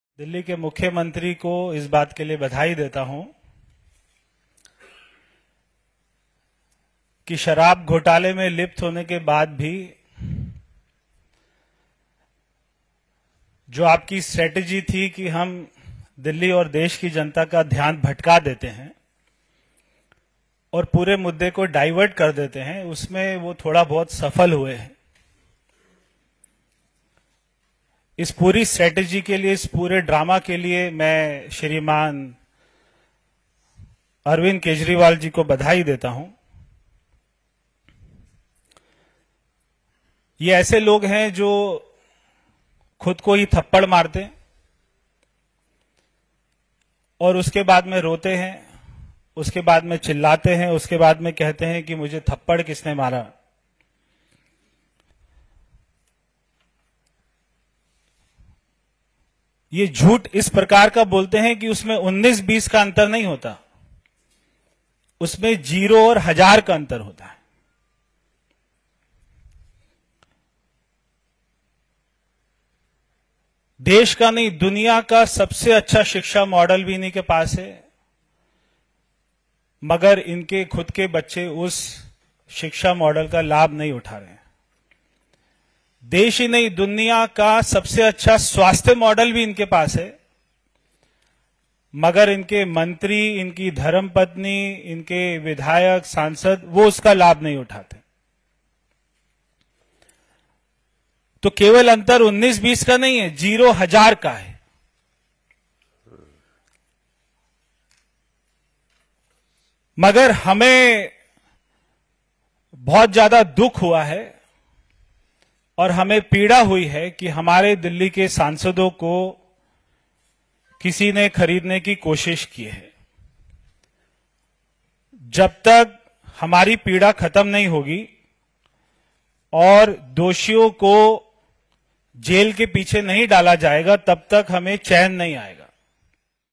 जब तक विधायक खरीद-फरोख्त करने वाले दोषियों को जेल में नहीं डाला जाएगा तब तक हमें चैन नहीं आएगा
केजरीवाल जी कहते हैं कि दुनिया का सबसे अच्छा शिक्षा और स्वास्थ्य मॉडल इनके पास है, मगर उनके बच्चे, परिवार, विधायक और सांसद उसका लाभ नहीं उठाते हैं।

- श्री @p_sahibsingh